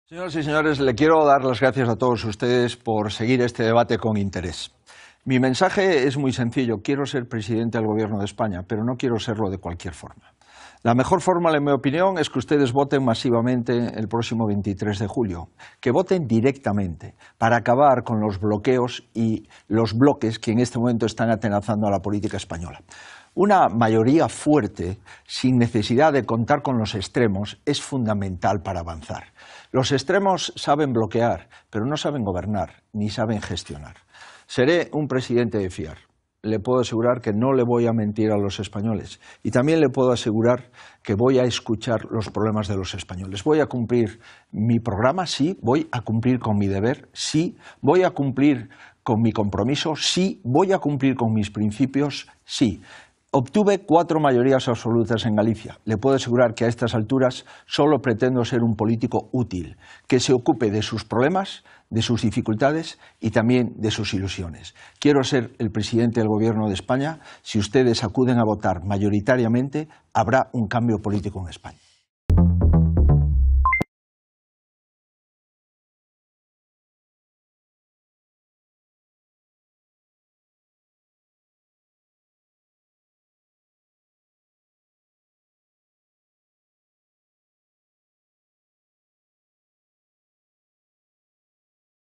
DEBATE 23J | El minuto de oro de Feijóo en el cara a cara con Sánchez | EL PAÍS
Así ha sido el minuto de oro de Alberto Núñez Feijóo, el candidato del PP a las elecciones generales.